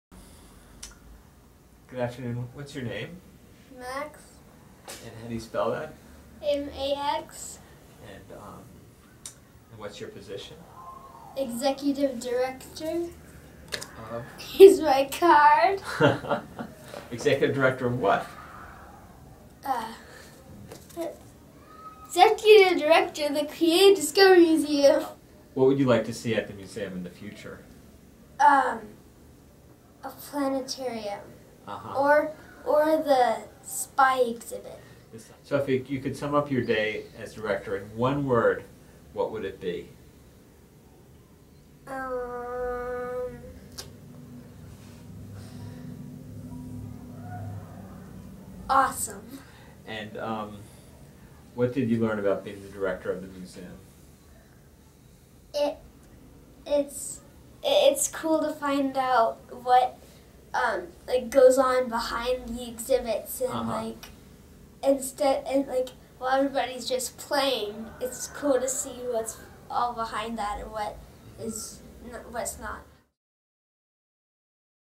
Creative Discovery Museum Director for the Day 2014
Say hello to Max Montague! Max recently got to be Director of the Creative Discovery Museum for the Day. See what he has to say about his experience in this exclusive interview.